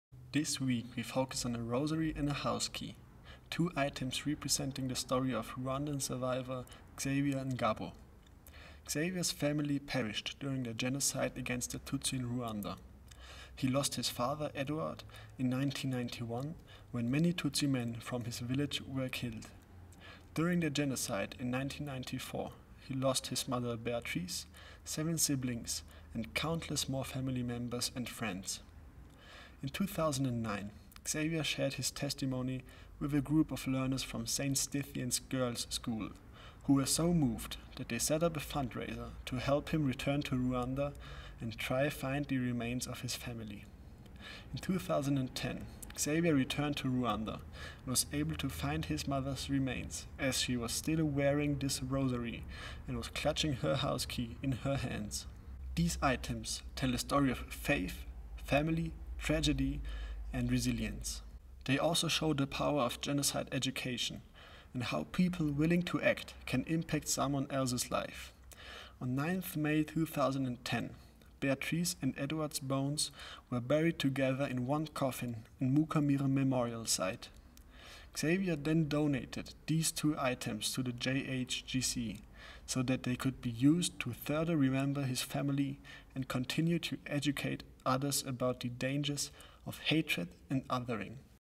Rosary and House Key | Archive Friday
This ArchiveFriday we focus on a rosary and a house key, two items representing the story of Xavier Ngabo, a survivor of the 1994 genocide against the Tutsi in Rwanda.

During the genocide, Xavier lost his mother Beatrice, seven siblings and countless more family members and friends. Xavier donated these two items to the JHGC in remembrance of his family, and to help educate about the dangers of hatred and othering. These items can be found on display in the JHGC's permanent exhibition.